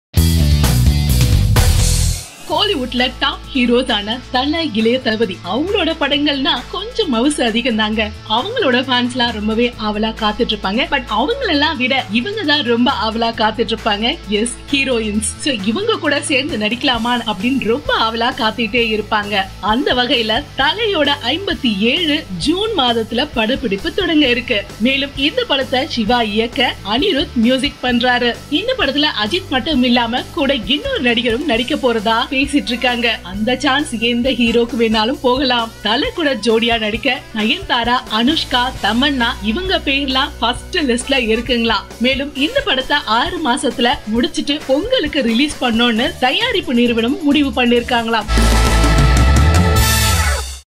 Ajith 57th Shooting Starts In June | Latest Tamil Movies News 2016
Kollywood Super Star "Ajith" 57th Shooting Starting In June.